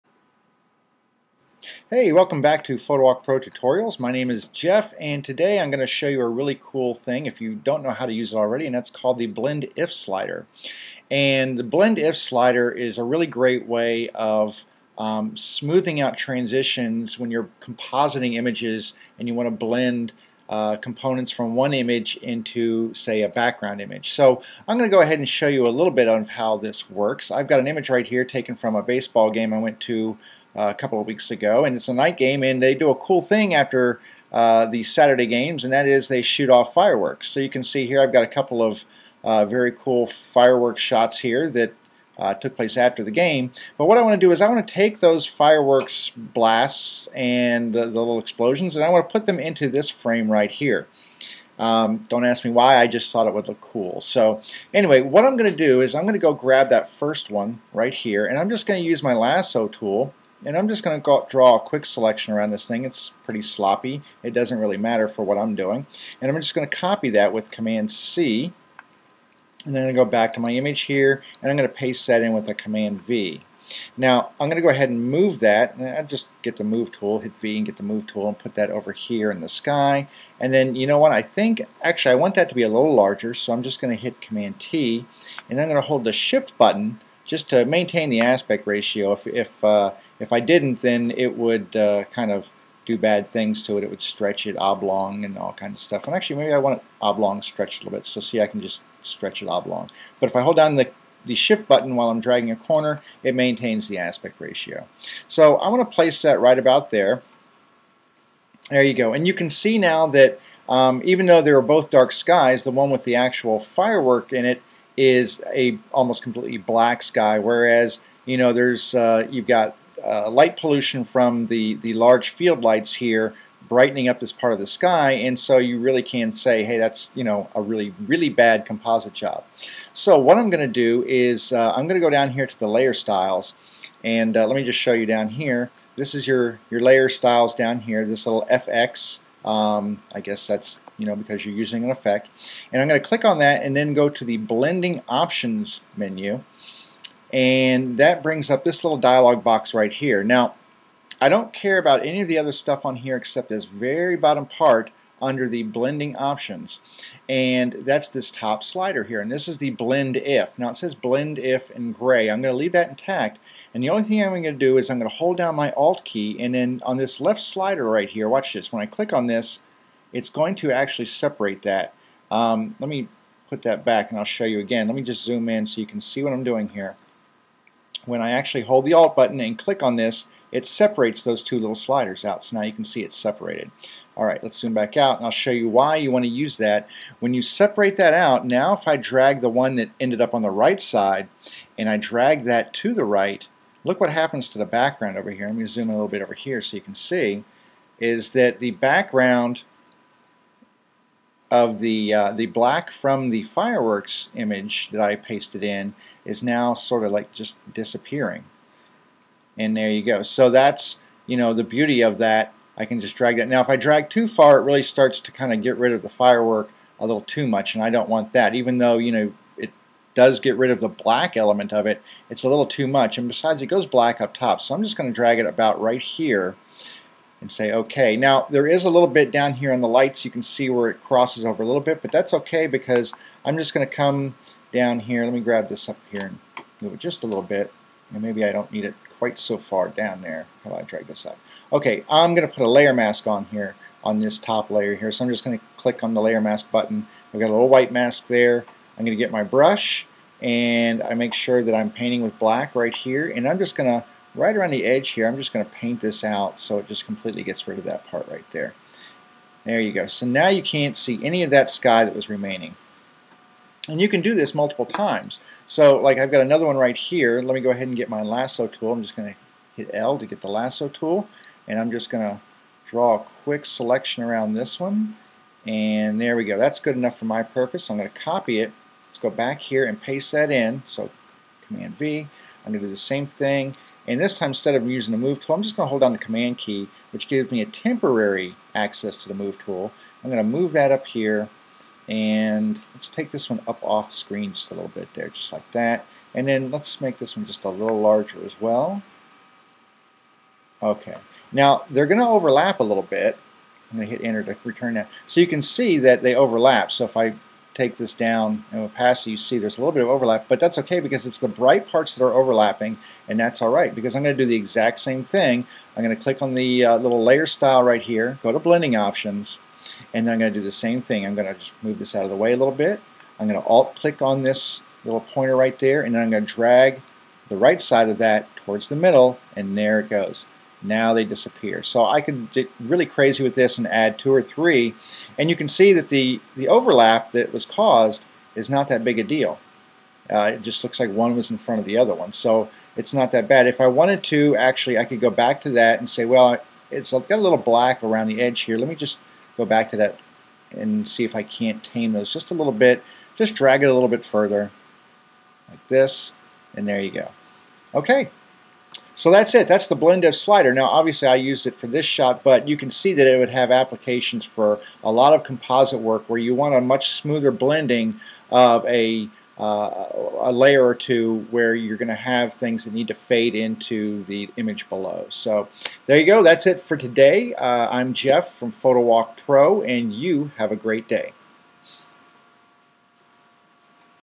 Blend If
Tutorial showing how you can use the blend-if slider in Photoshop to make a better composite image.